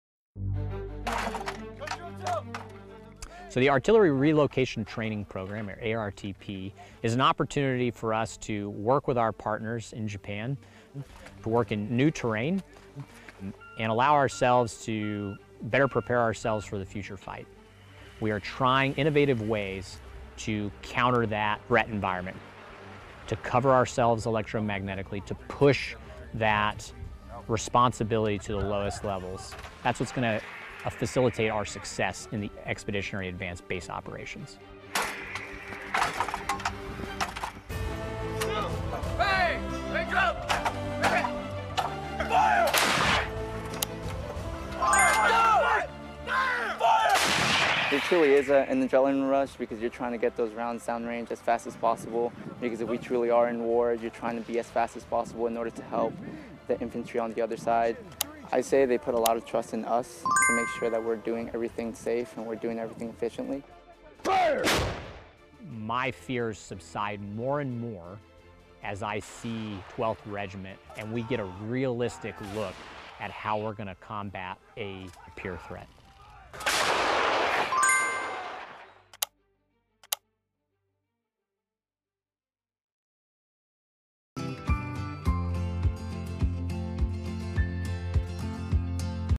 3d Battalion, 12th Marines ARTP 21.3
U.S. Marines with 3d Battalion, 12th Marines, 3d Marine Division, conduct Artillery Relocation Training Program 21.3 (ARTP) at Yausubetsu Maneuver Area, Hokkaido, Japan, Oct. 22-31, 2021.

3d Battalion, 12th Marines ARTP 21.3
YAUSUBETSU MANEUVER AREA, HOKKAIDO, JAPAN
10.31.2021

ARTP is an exercise held to strengthen the defense of Japan and the U.S.-Japan Alliance as the cornerstone of peace and security in the Indo-Pacific region. This iteration of ARTP focused on operating as a small, distributed force, capable of delivering long range precision fires with lethal effects from multiple survivable locations. (U.S. Marine Corps video by Sgt. Kirstin Spanu)

Film Credits: Video by Sgt. Kirstin Spanu
3rd Marine Division